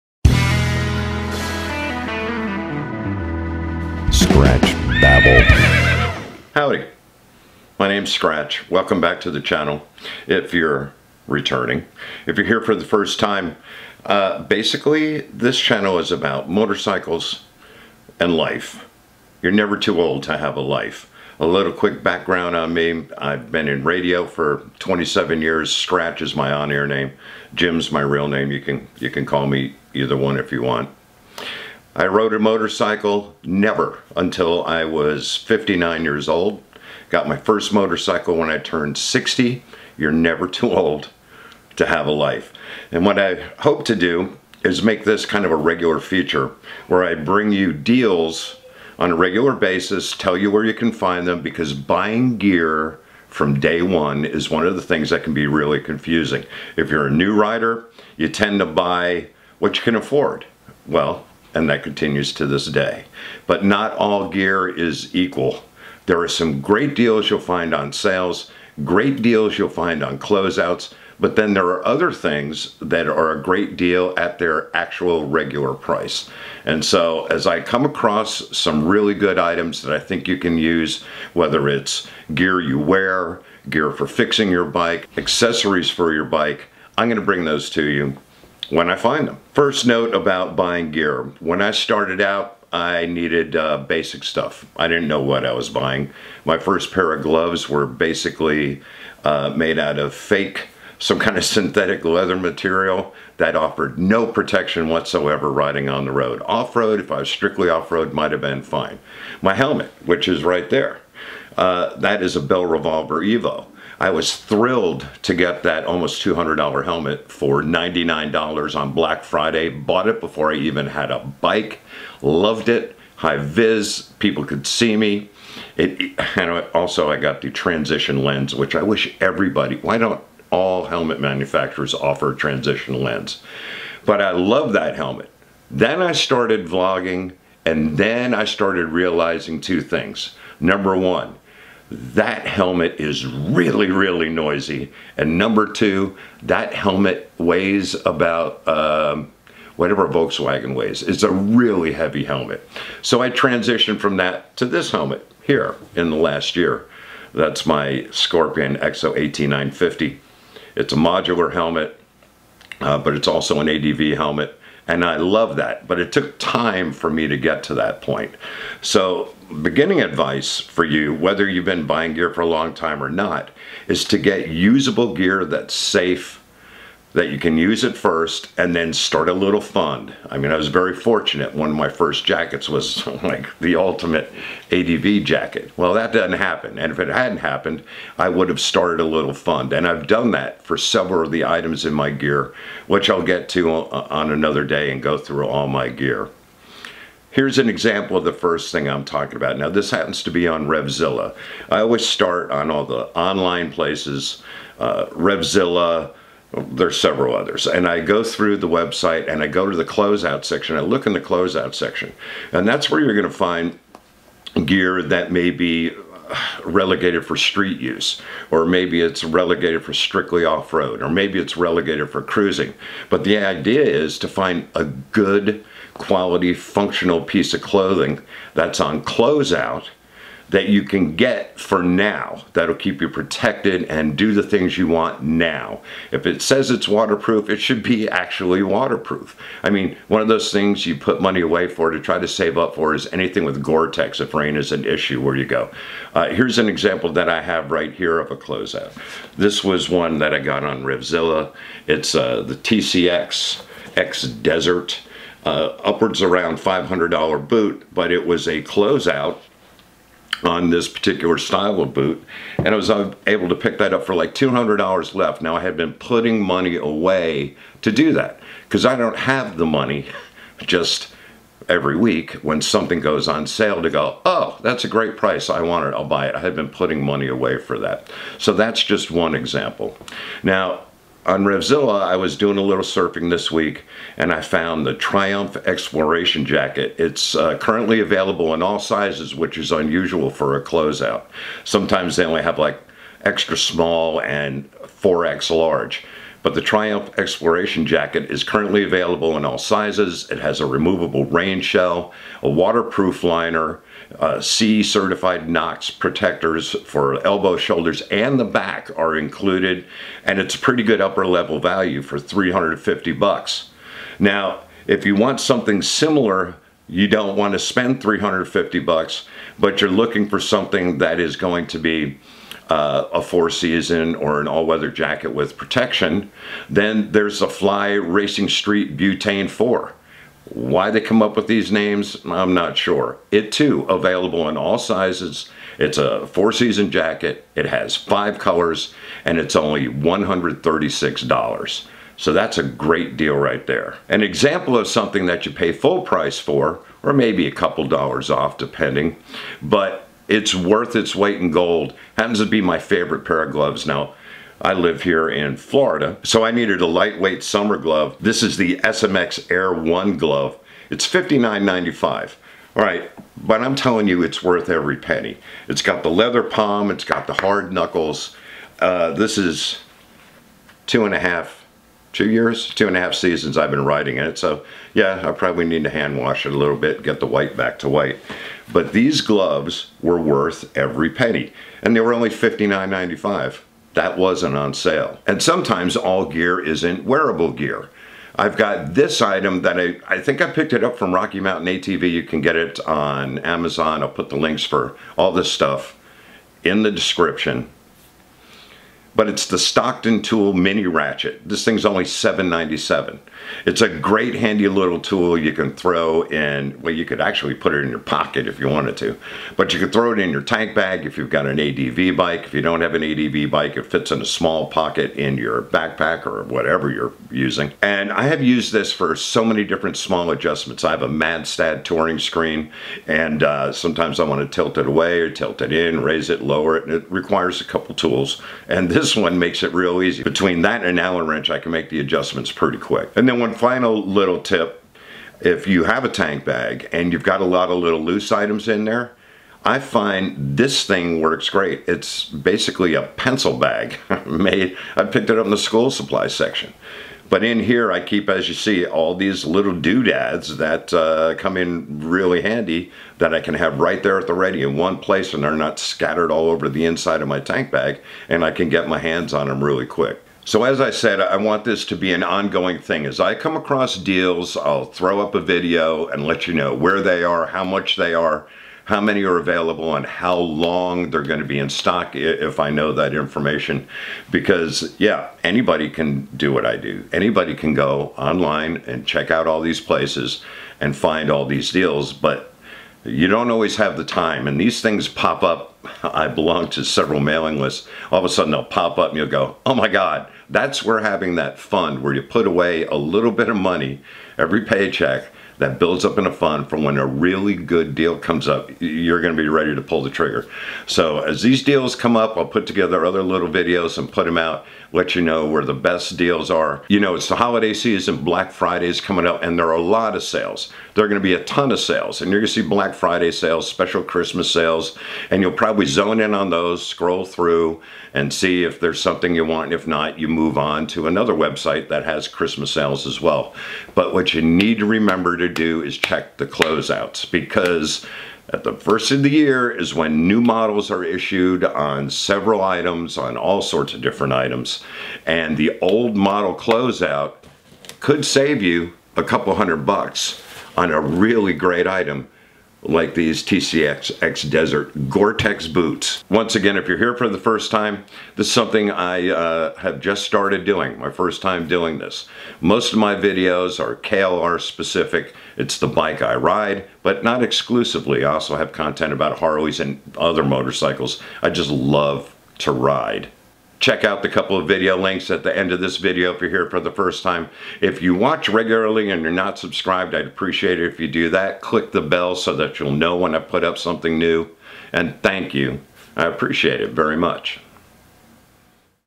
How to shop for motorcycle gear
How I shop for gear PLUS some deals I just found.

Scorpion Exo AT950 Neocon helmet

Klim Badlands Pro Jacket

Alpinestars SMX-1 Air Men's Street Motorcycle Gloves

STOCKTON TOOL COMPANY Mini Ratchet Kit

TCX X-Desert GTX Men's Street Motorcycle Boot

Helmet
Jackets
Rev’it Eclipse Jacket
Pants
ARC Battleborne Pants
Gloves
Scorpion TSW gloves (cold weather)
Boots
TCX Street Ace WP (brown)
Icon Patrol (best discontinued short boot EVER)
Mid layers
Klim Inferno Jacket